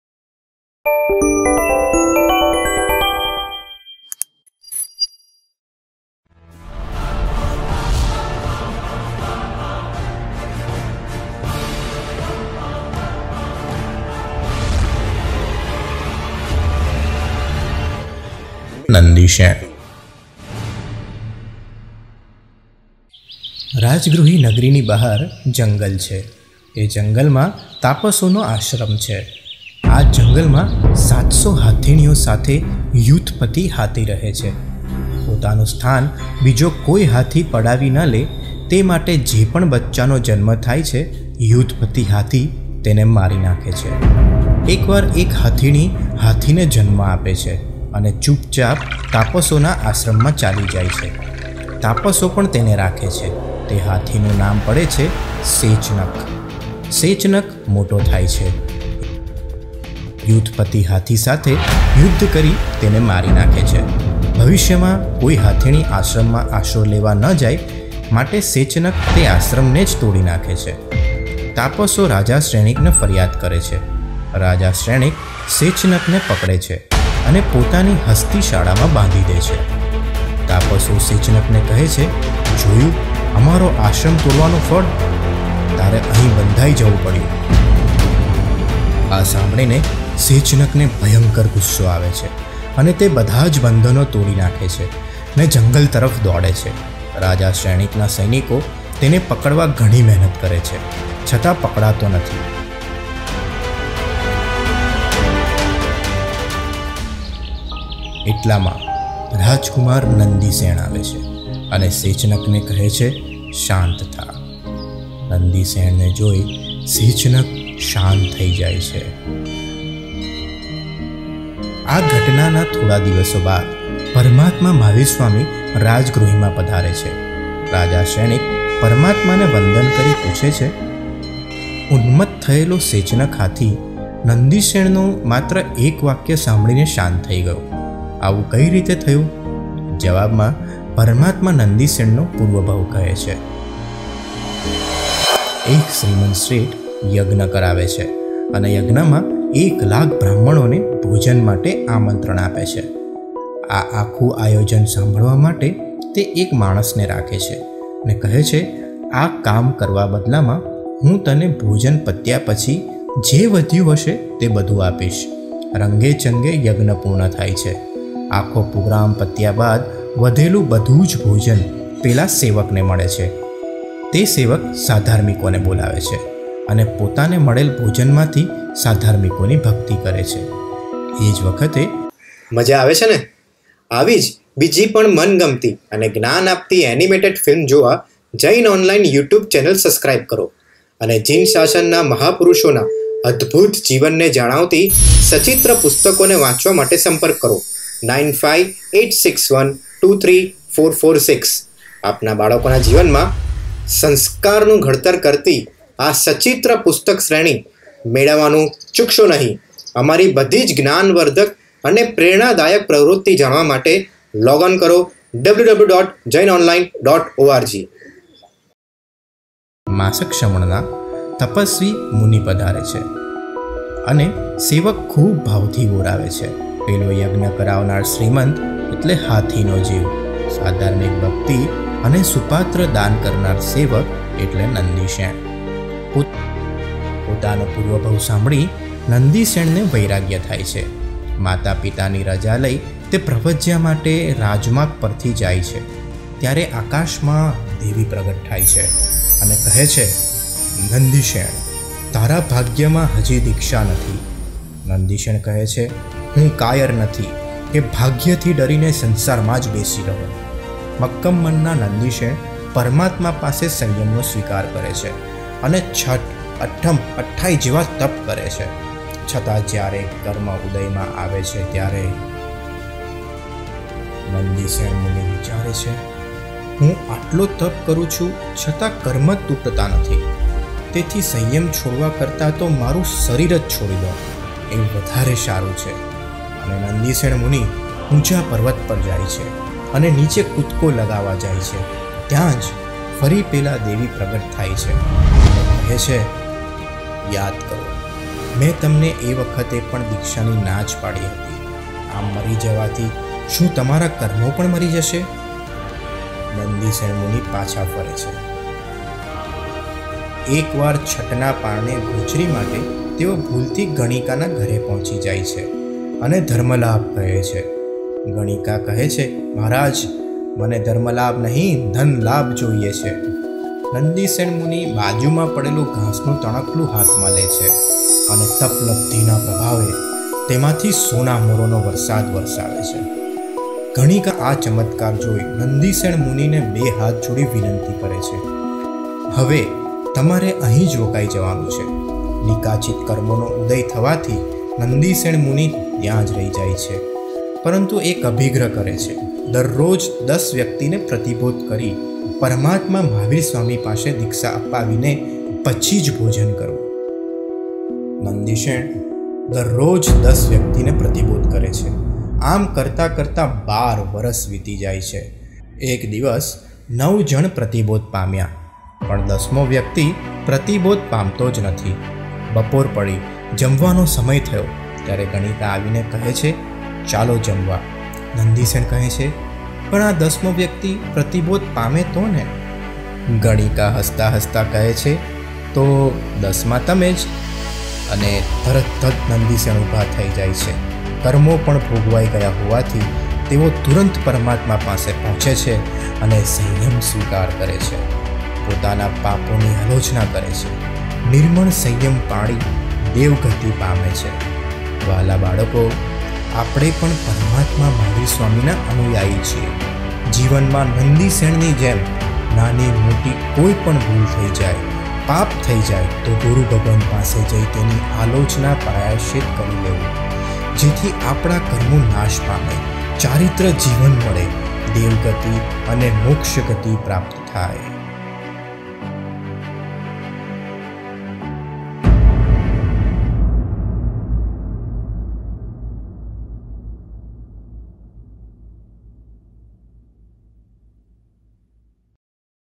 Nandishen | Jain Gujarati Animation Children Story for kids | To buy Story books
A set of 25 books on Ideal personalities of Jainism available at attractive price. Available in Gujarati, Hindi  and English. Click to Buy

Get to know the jain children story of Nandishen Muni  through this animated video and Jain gujarati story for kids.

This is one of the animated jain story for kids in the series of "Jinsashan na Mahapurusho" based on pictorial stories written by Param Pujya Acharya Shri Bhuvanbhanu suri Maharaj Saheb

for more such videos